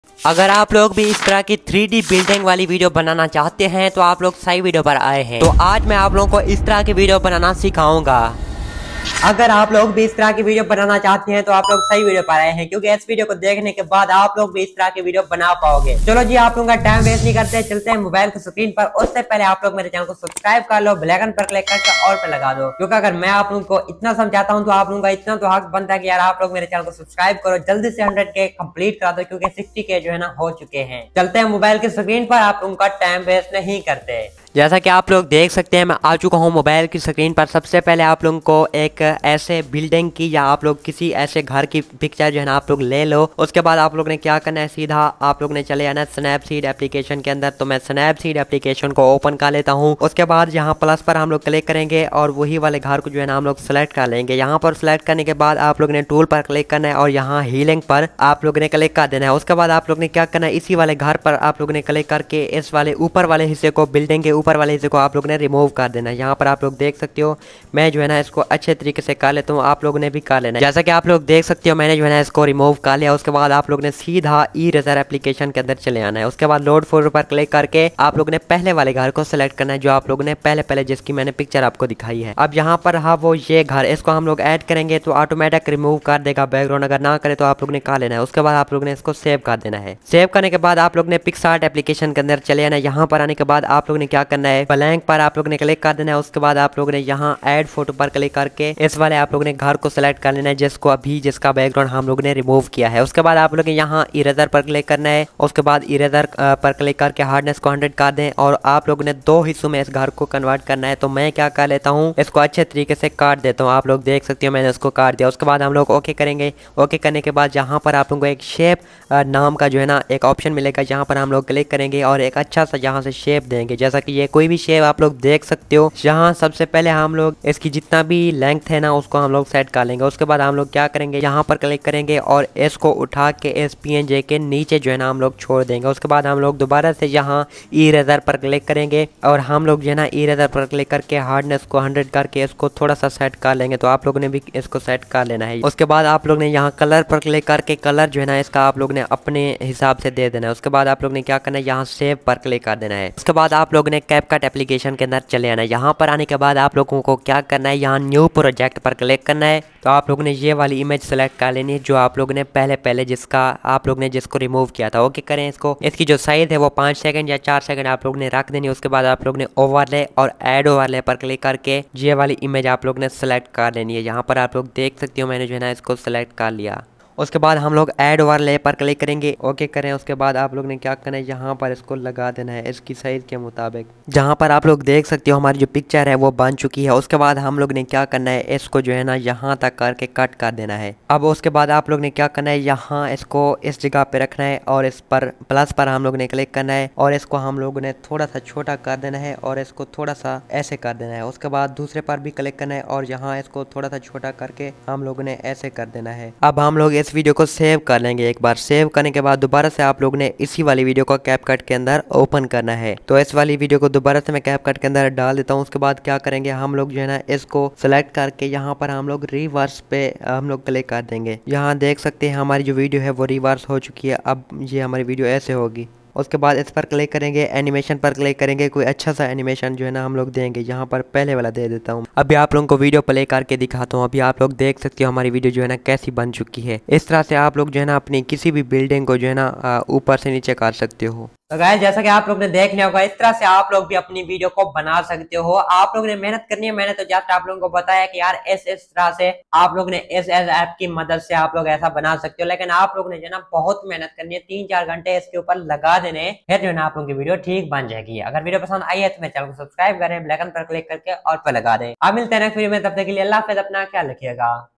Trending New Building 3D effects Reels Tutorial | Capcut Trending 3D building Editing 2024
Assalamualaikum friends
i hope you are all fine today😘
In this video of today we will learn how to Assemble...😍

Ragards :
Jam_Imran_Official